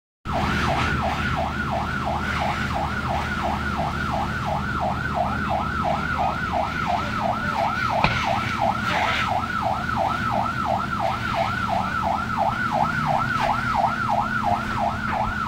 Incendiu de vegetaţie în Podari
În urmă cu puţin timp a izbucnit un incendiu în zona Balta Verde din comuna Podari.